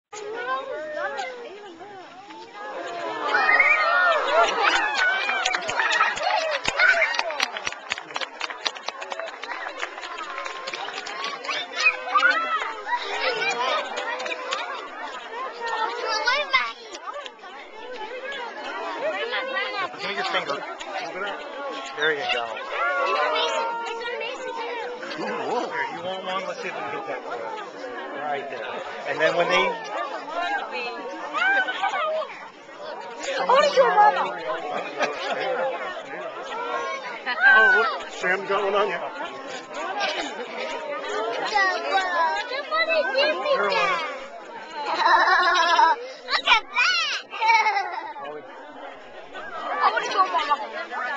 Germanna Community College hosts butterfly release as Hospice of the Rapidan fundraiser
The children, grandchildren and great grandchildren of patients at the Hospice of the Rapidan enjoy the release of 200 Monarch Butterflies during a charity fundraiser at Germanna Community College in Virginia.